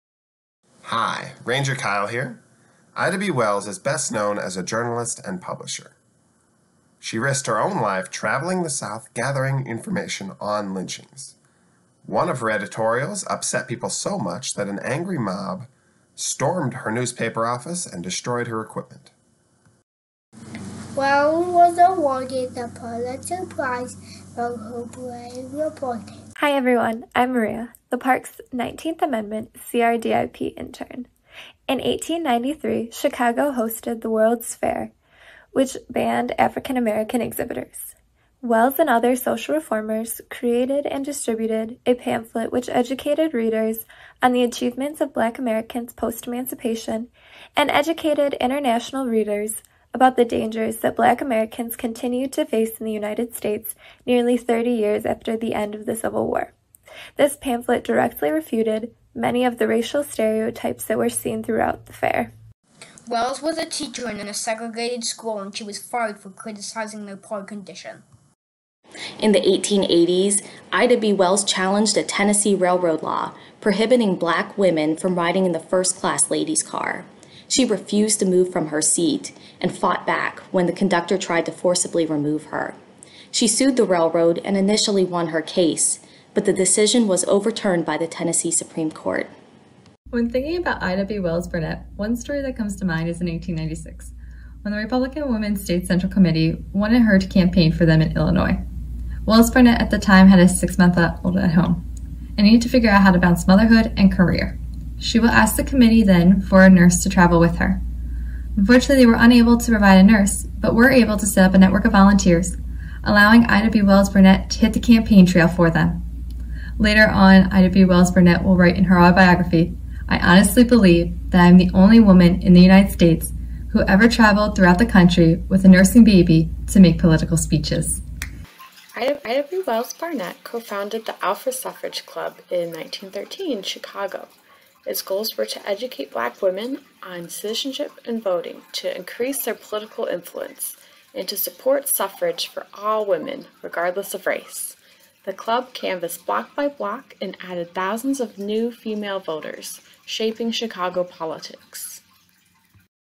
Ida B. Wells-Barnett
Ida B. Wells, Ida B. Barnett, or Ida B. Wells-Barnett? What is in a name?

When Ida B. Wells married Ferdinand Lee Barnett in 1895, Wells wanted to maintain her identity as an accomplished reporter and activist. So instead of following traditional norms of the 19th century in adopting his last name, she hyphenated her last name becoming Ida B. Wells-Barnett.

Find out more about Ida B. Wells-Barnett from our Rangers in this week's video.